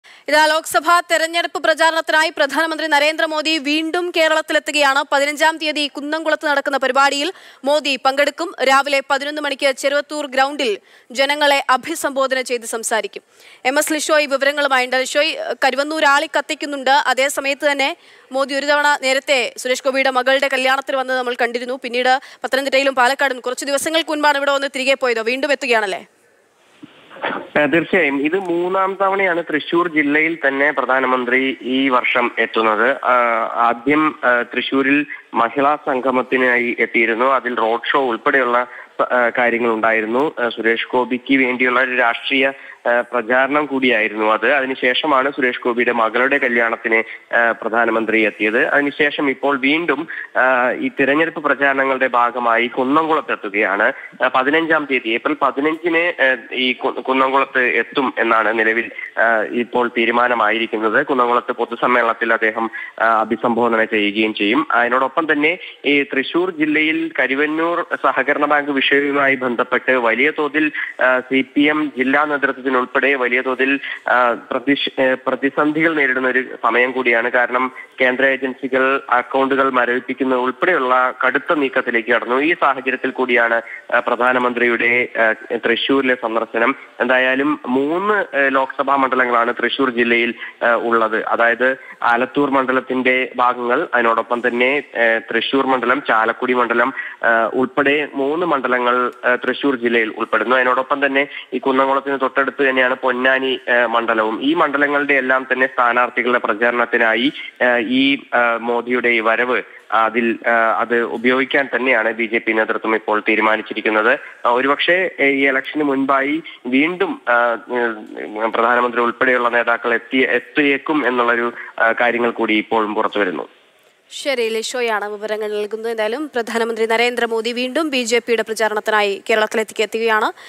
നരേന്ദ്ര മോദി വീണ്ടും കേരളത്തിലേയ്ക്ക്; ഏപ്രിൽ 15ന് കേരളത്തിലെത്തും | P M Modi
ലോക്സഭ തിരഞ്ഞെടുപ്പ് പ്രചാരണത്തിനായി പ്രധാനമന്ത്രി നരേന്ദ്രമോദി വീണ്ടും കേരളത്തിലേയ്ക്ക്

Happy viewing!